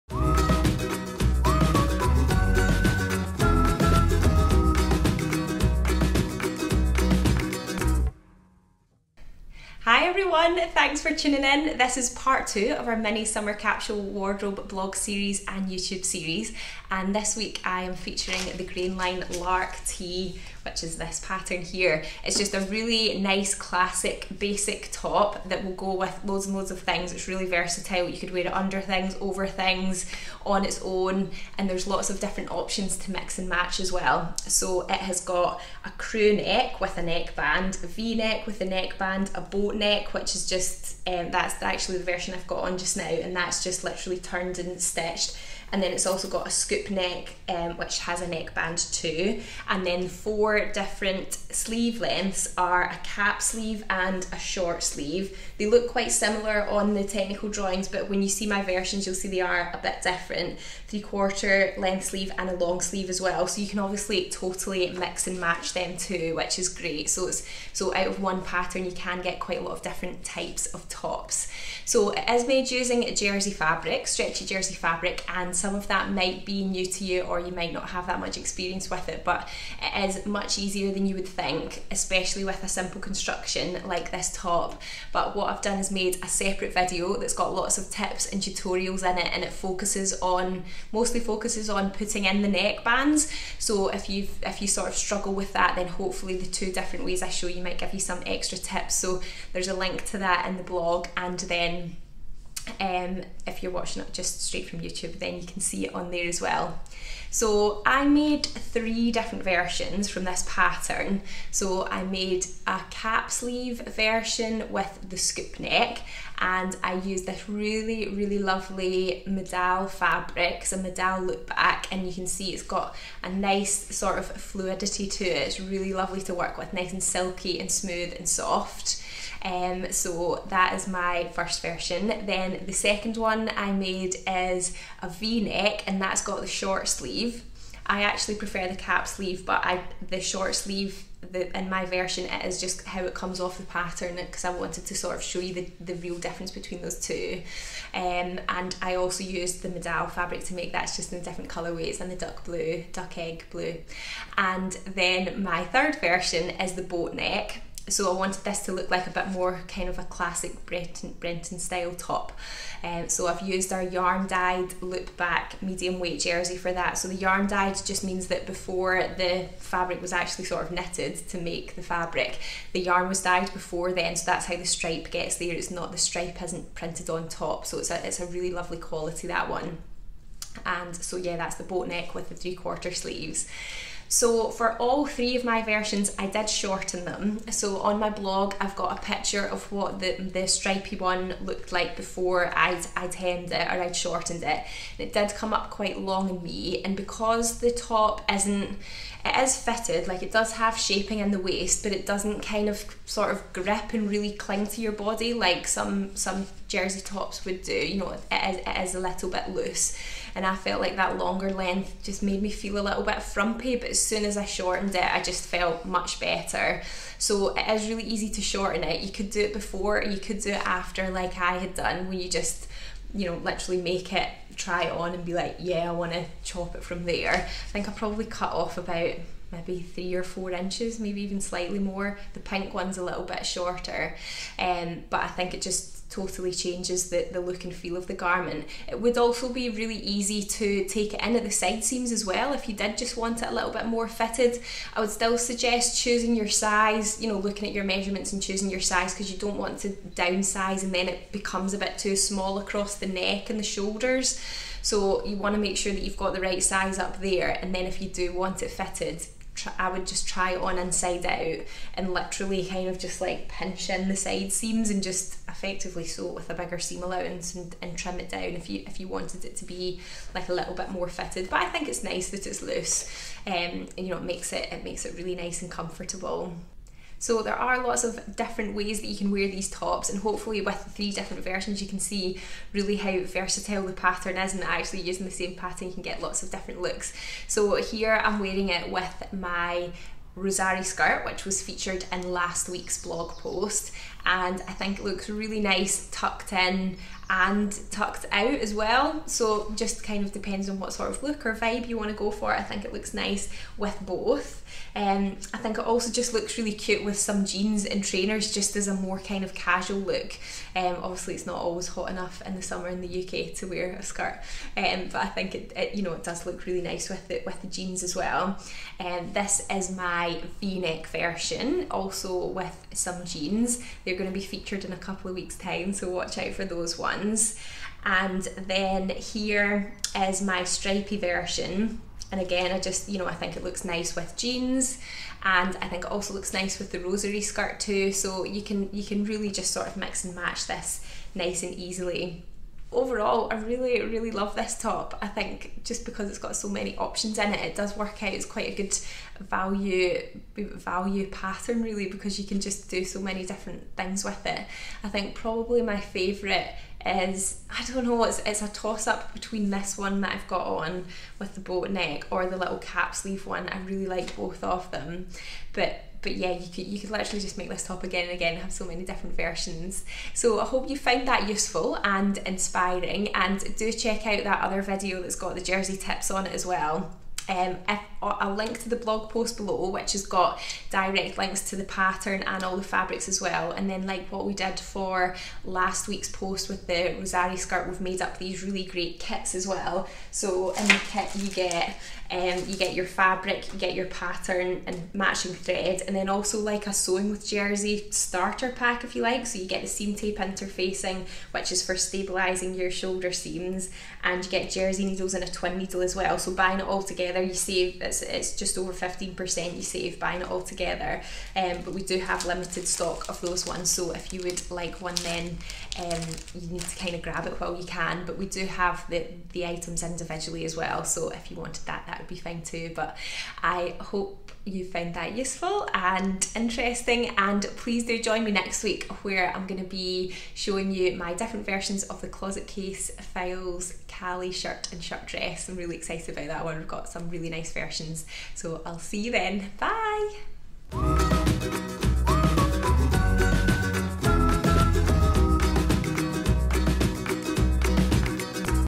Lark tee - Mini Summer Capsule Wardrobe Series - Part 2
It features the Grainline patterns Lark tee and you can find out more in this blog post including links to the pattern, fabrics and our limited edition kits!